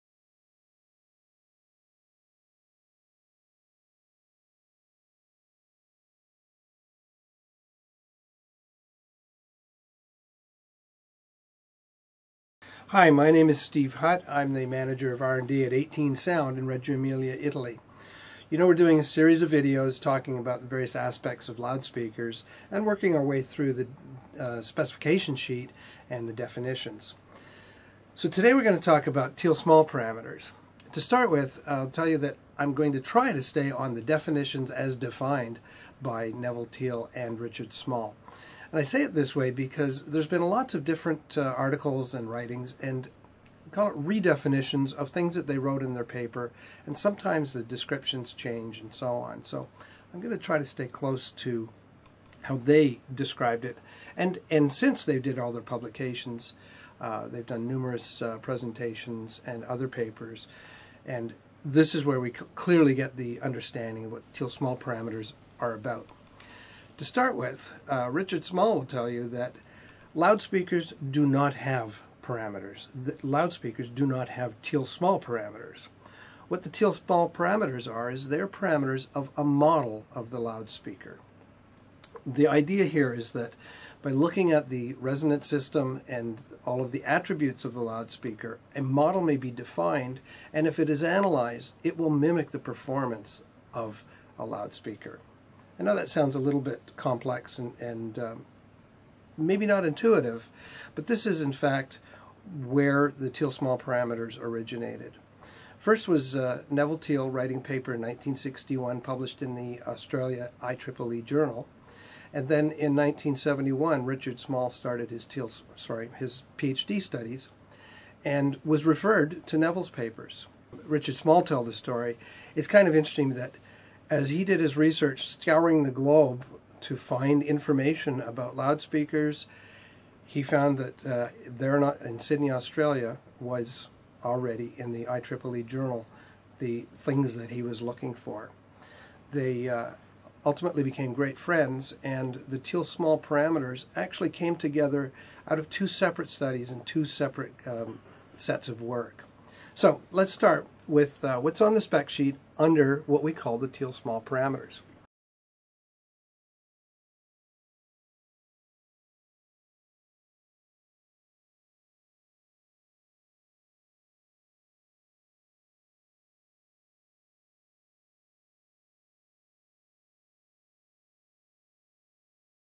18Sound - Loudspeaker Lyceum - Chapter 4 - Introduction - The Thiele Small Parameters
In this video 18Sound R&D Manager Steve Hutt explains how Thiele Small parameters originated .